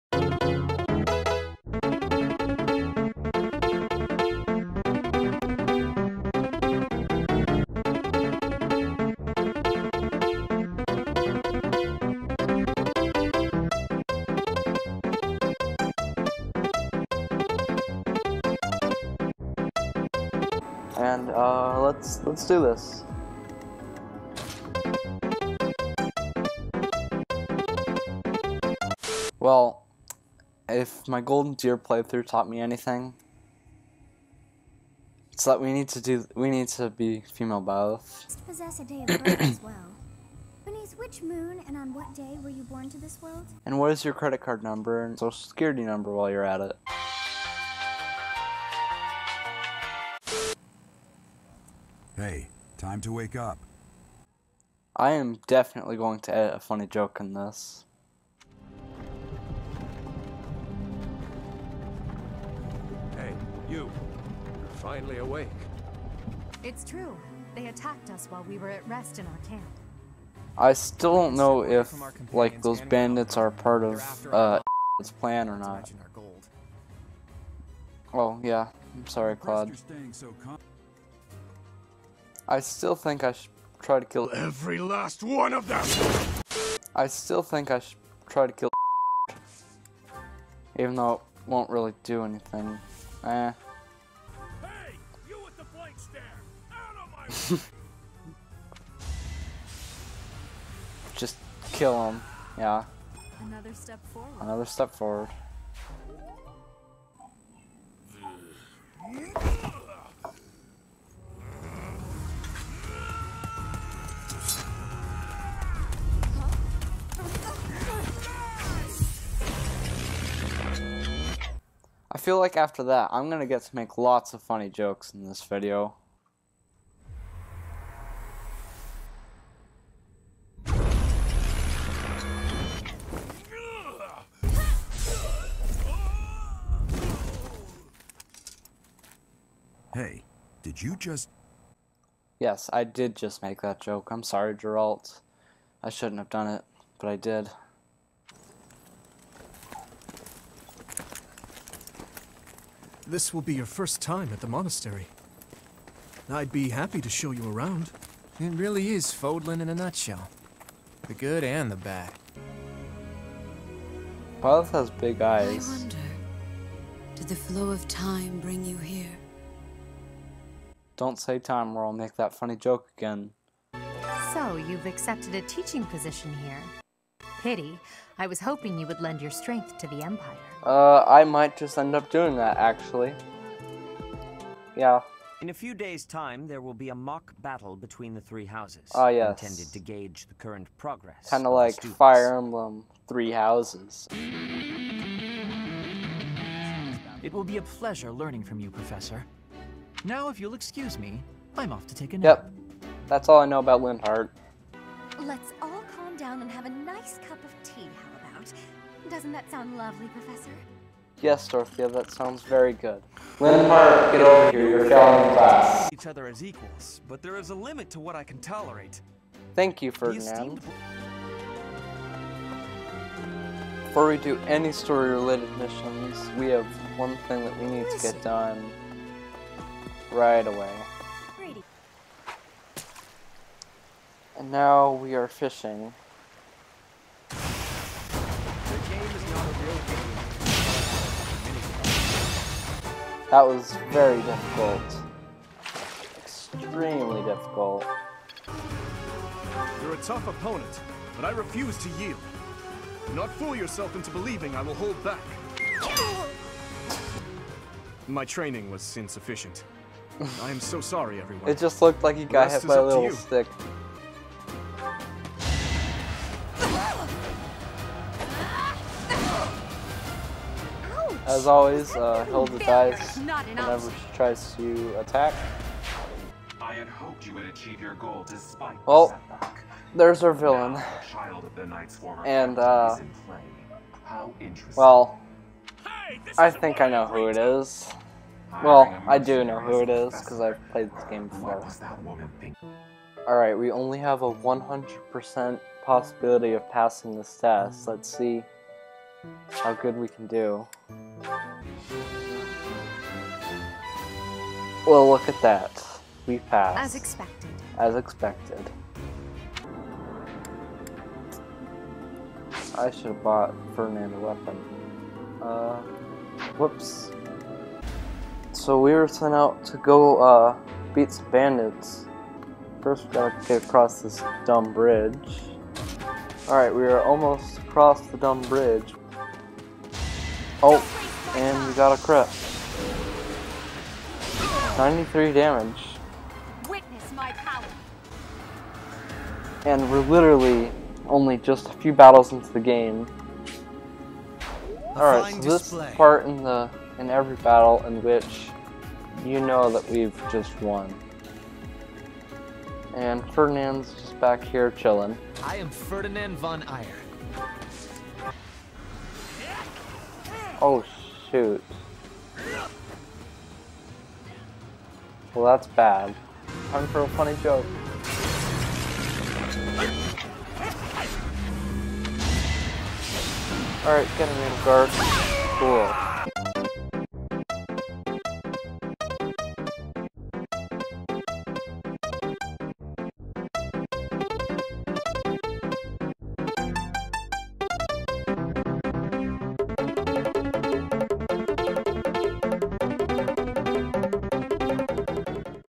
Why I Like to Actually Edit My Videos (Pilot Episode)
This video is the reason why I like it when my videos are actually edited to what I call a "higher standard"